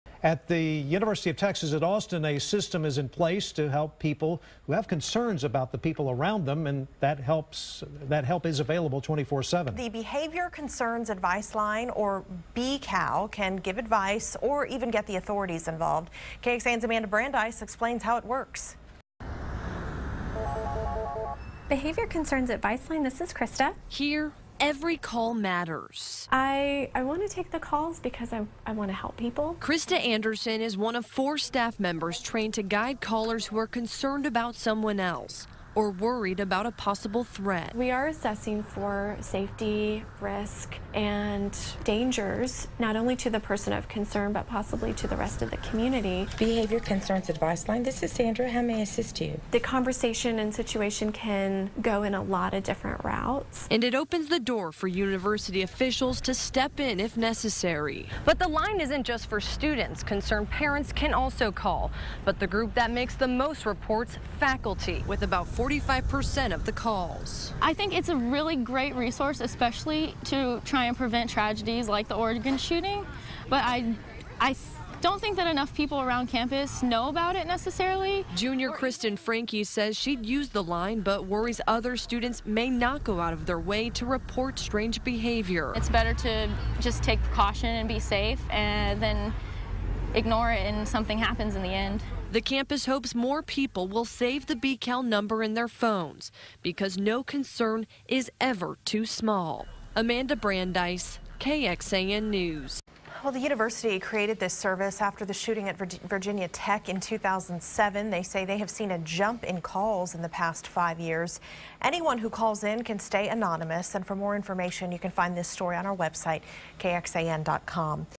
System in place at UT Austin to help people with behavior concerns
The Behavior Concerns Advice Line can give advice, or get the authorities involved